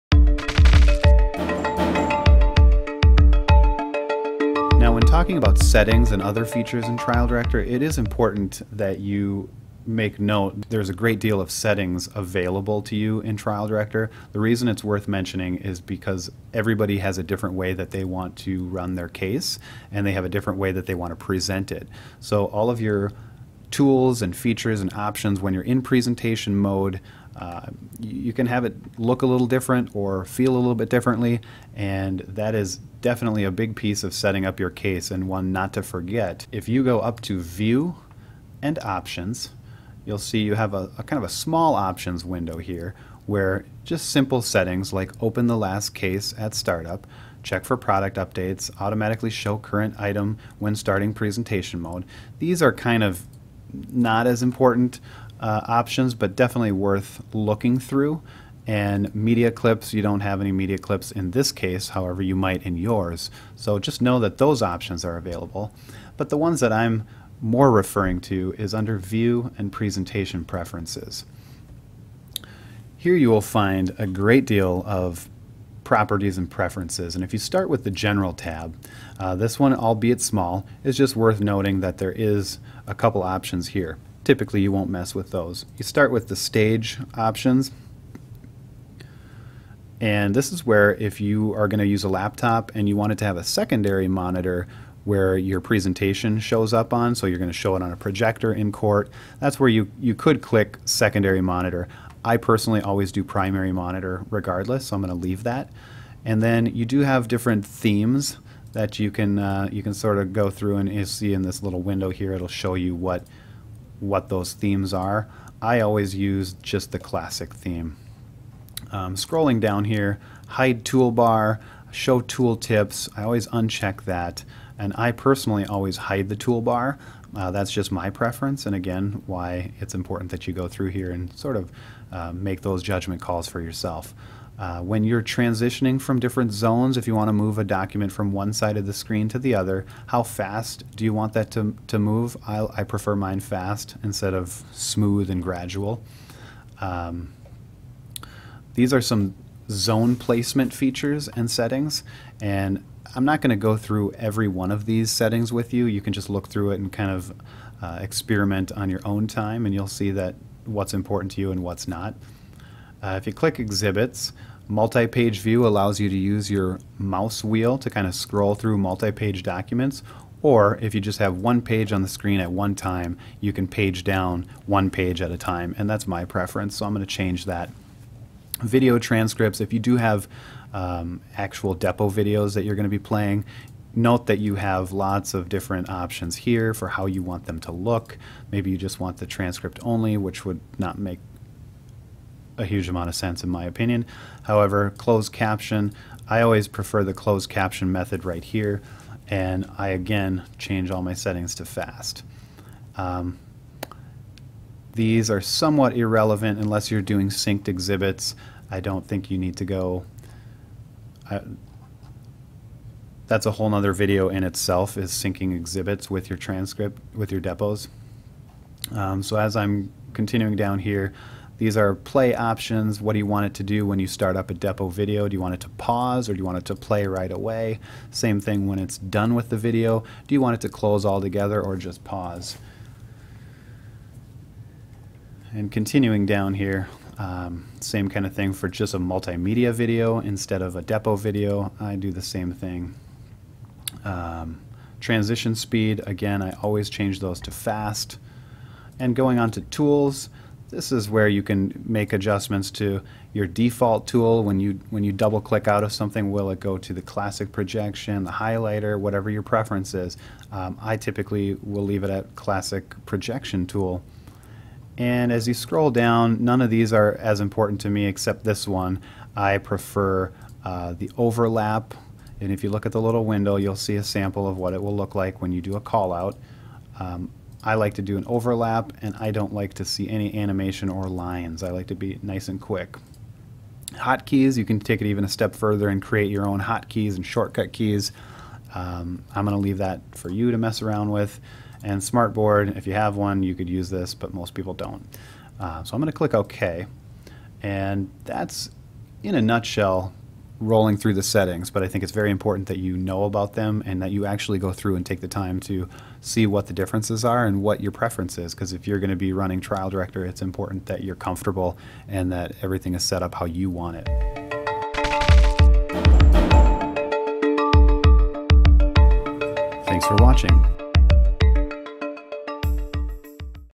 Trial Director - Step 3 Understanding Settings & Preferences
A quick overview of the settings and preferences within Trial Director